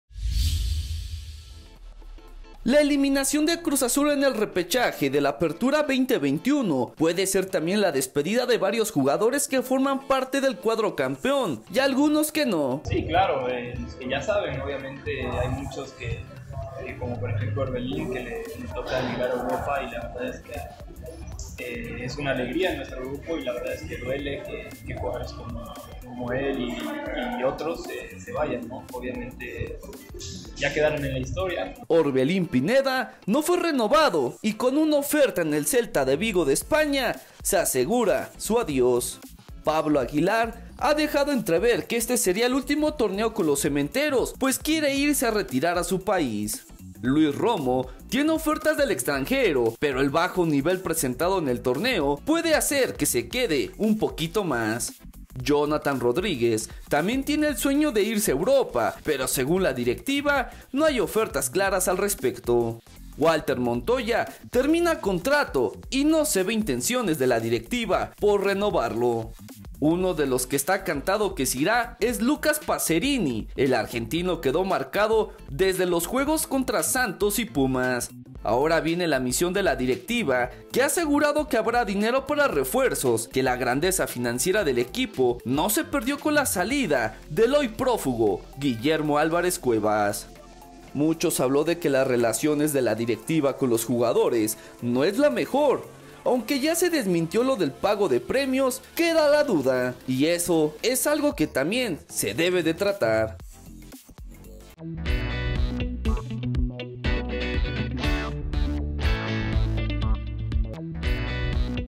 La Máquina en riesgo de perder a futbolistas importantes
Tras fracasar en el Apertura 2021, el Cruz Azul tendría bajas considerables para el siguiente torneo